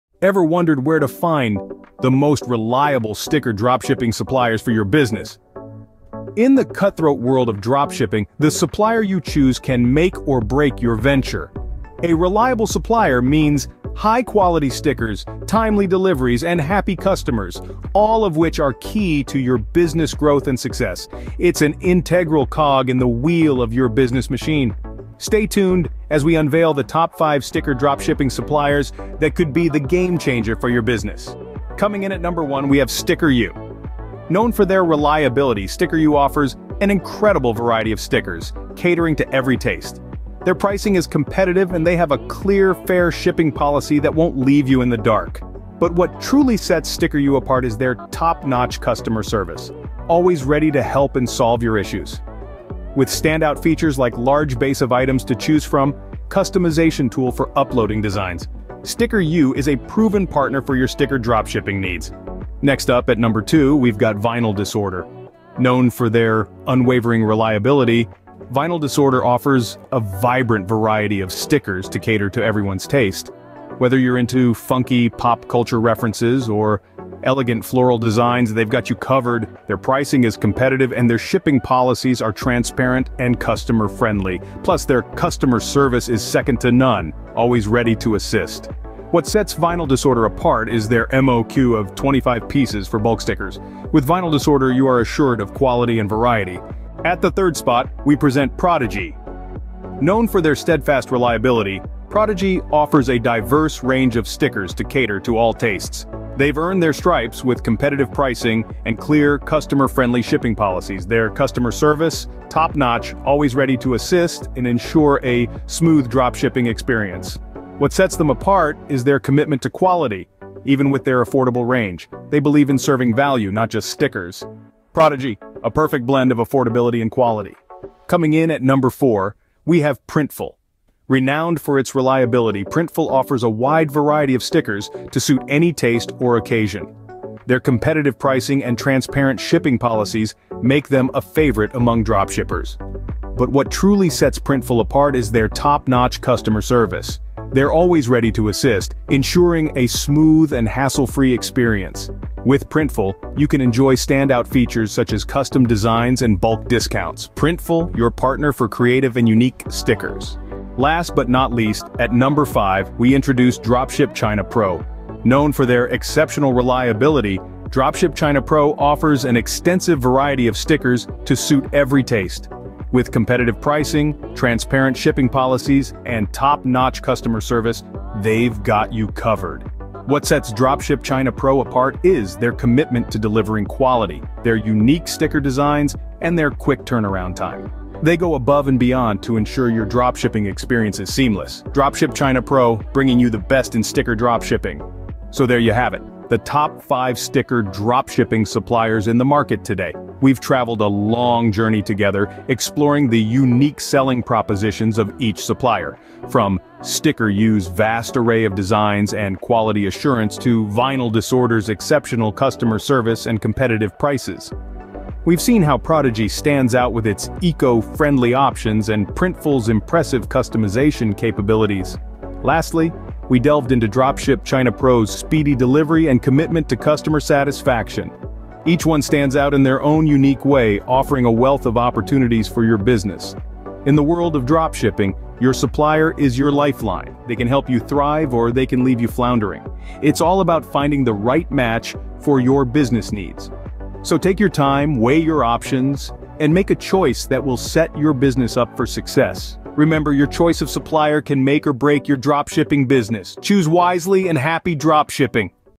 Top 5 Stickers Dropshipping Suppliers
In this video, we unveil the Top 5 Stickers Dropshipping Suppliers that are perfect for dropshipping entrepreneurs. A premier stickers dropshipping supplier, offering a wide variety of customizable stickers for personal or business use with fast shipping and competitive pricing.